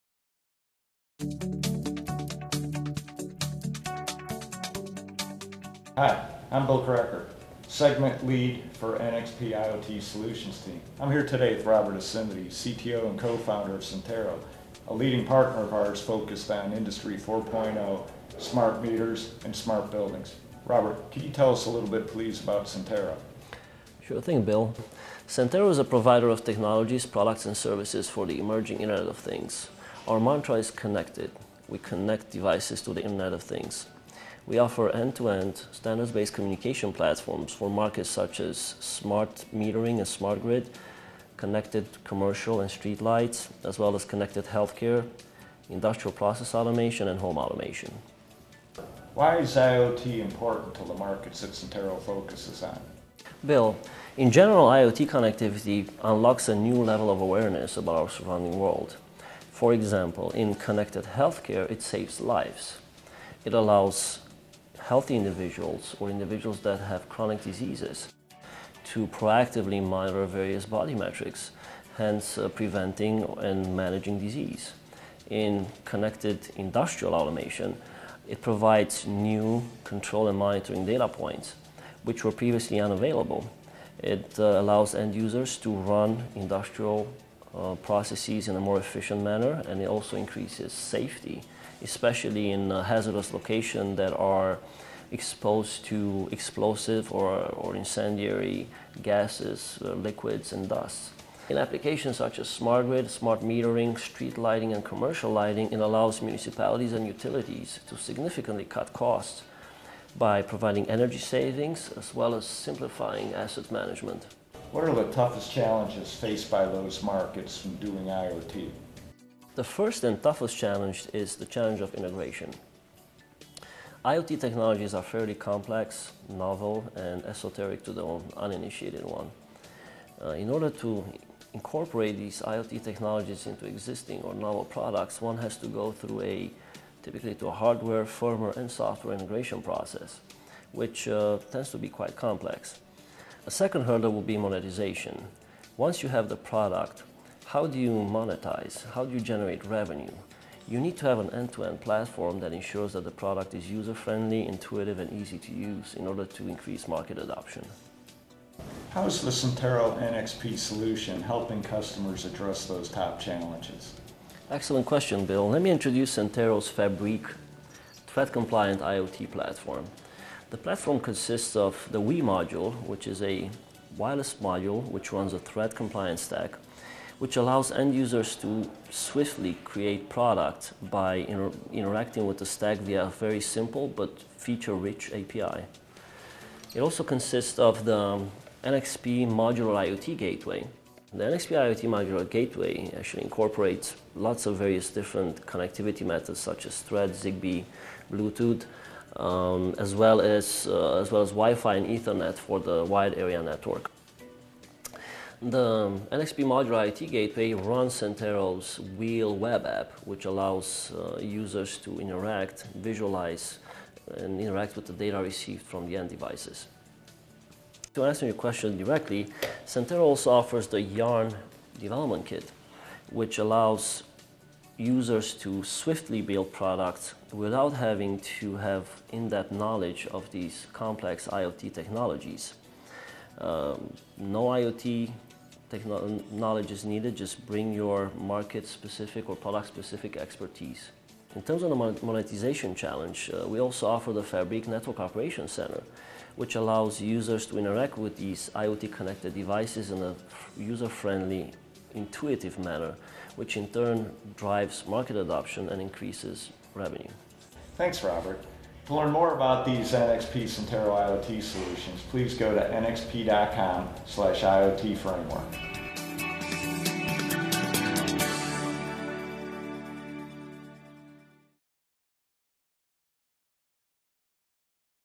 NXP and Centero discuss IoT Solutions for Industry 4.0, Smart Meters and Smart Buildings.
Learn how NXP and Centero are leveraging the Modular IoT Gateway to help customers with their IoT development challenges.